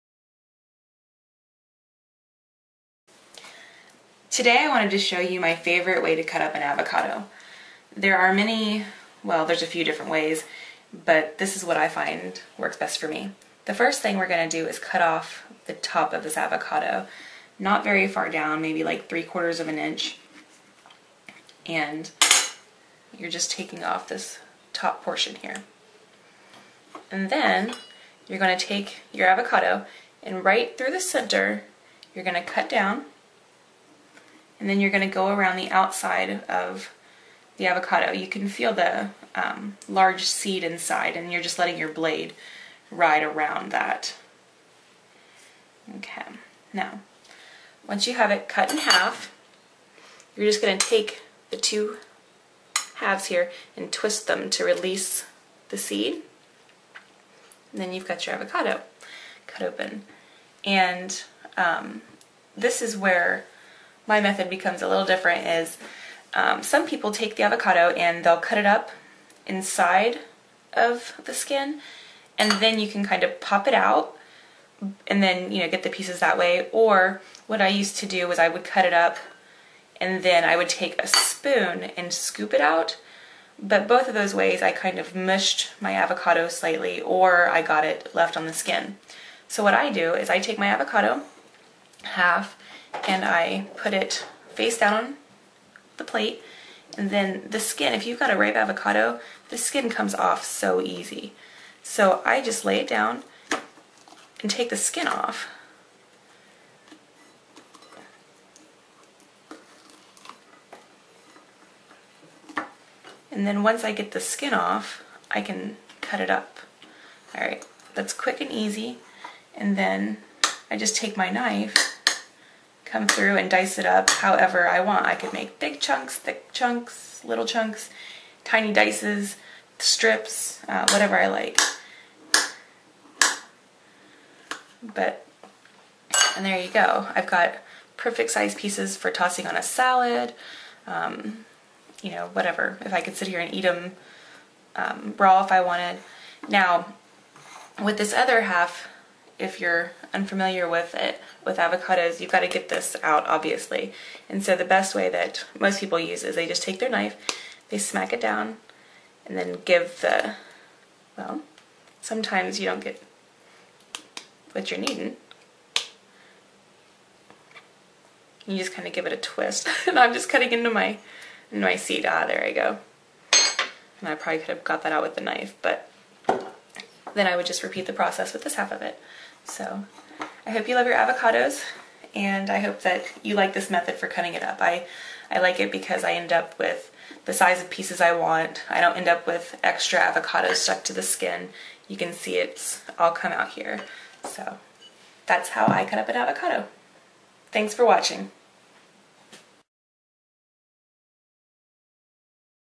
How To Cut Up An Avocado
This is my favorite way to cut up avocados.  This is great for cutting up your avocado into pieces, strips or whatever you might like for sandwiches, salads, etc.  I hope you like it too!

If you only need half of the avocado, you can store the other half in an avocado saver

If you are just using your avocado for something like guacamole, then you could easy scoop it out with a spoon.